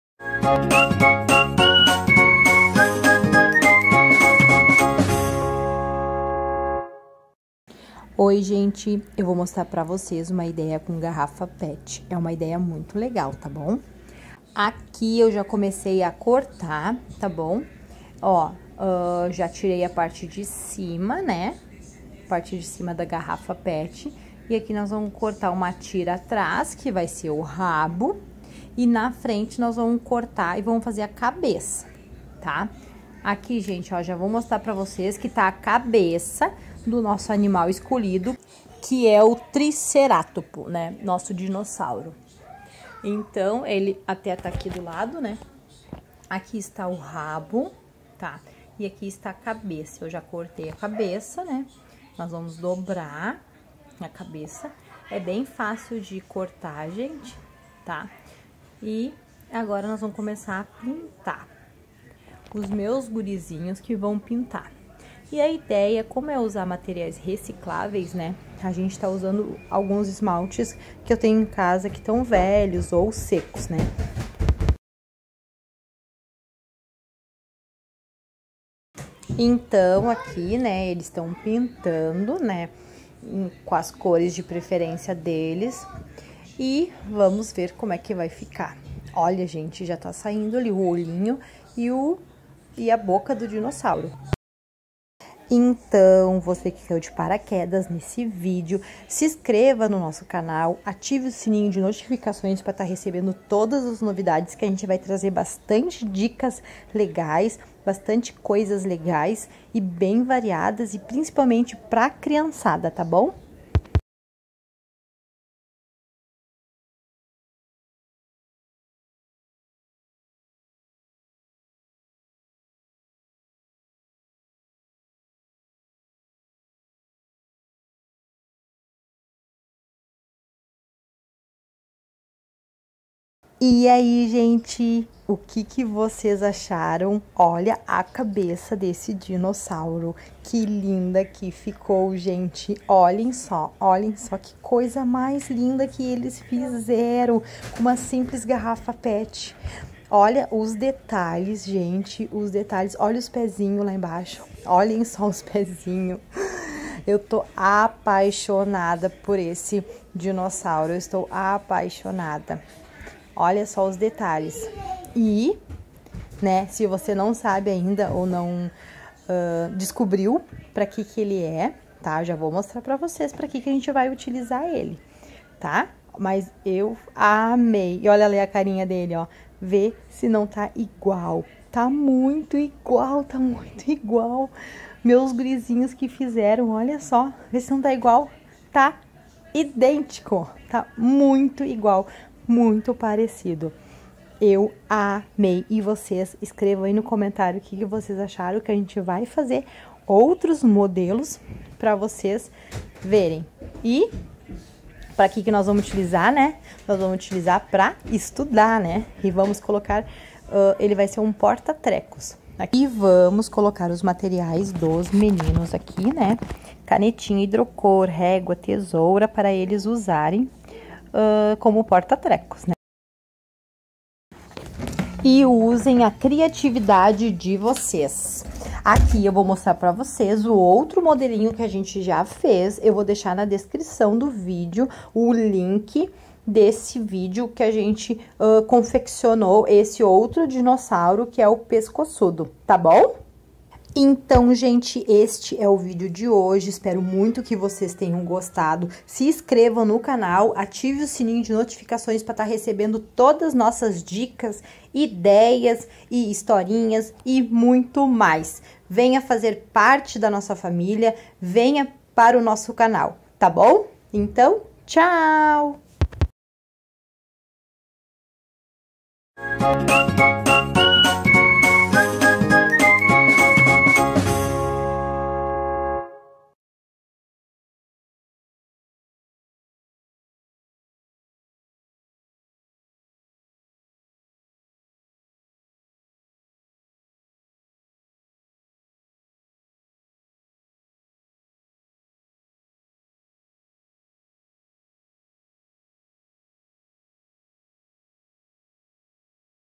Garrafa pet Idéias I Dinossauro - porta trecos- Dino pet - reciclagem
Faça você mesmo um dinossauro de garrafa pet, modo simples, as crianças vão adorar, recicle e crie.